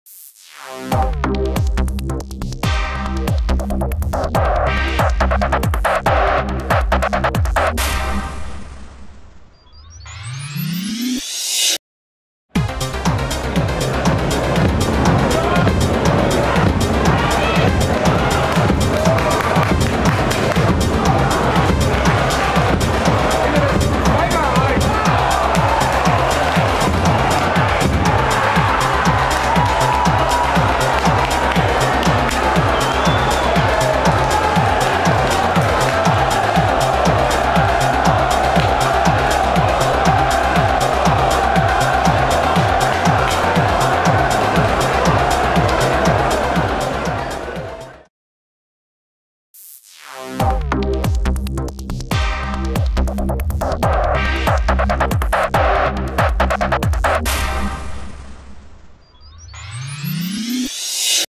[SVWW] KSV Hessen Kassel vs. SV Wehen Wiesbaden [SV Wehen Wiesbaden]
Das Tor von Maciej Zieba im Hessenpokal gegen Kassel. [präsentiert von der Spielbank Wiesbaden]